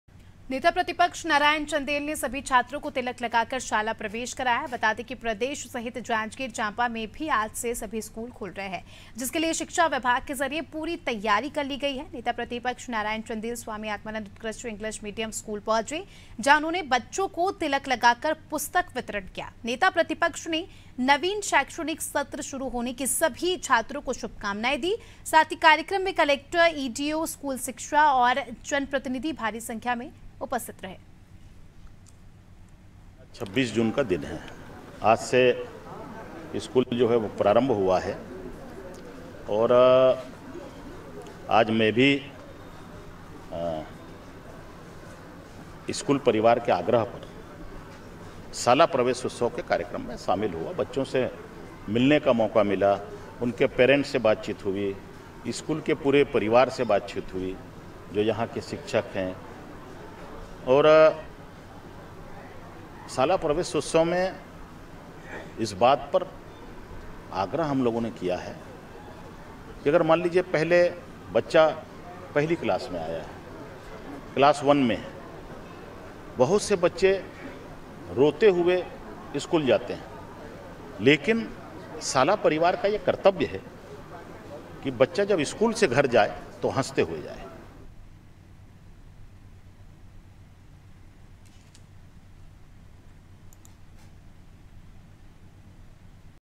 Janjgir-Champa : नेता प्रतिपक्ष ने बच्चों का कराया शाला प्रवेश | CG Hindi News || Anaadi Tv
नेता प्रतिपक्ष नारायण चंदेल ने सभी छात्रों को तिलक लगाकर शाला प्रवेश कराया...बता दें, प्रदेश सहित जांजगीर चांपा में भी आज से सभी स्कूल खुल रहे हैं...जिसके लिए शिक्षा विभाग के जरिए पूरी तैयारी कर ली गई है...नेता प्रतिपक्ष नारायण चंदेल स्वामी आत्मानंद उत्कृष्ट इंग्लिश मीडियम स्कूल पहुंचे...जहां उन्होंने बच्चों को तिलक लगाकर पुस्तक वितरण किया..नेता प्रतिपक्ष ने नवीन शैक्षणिक सत्र शुरु होने की सभी छात्रों को शुभकामनाएं दी...कार्यक्रम में कलेक्टर, डीईओ, स्कूल शिक्षक और जनप्रतिनिधि भारी संख्या में उपस्थित रहे.

COMING SOON
 DEN, AIRTEL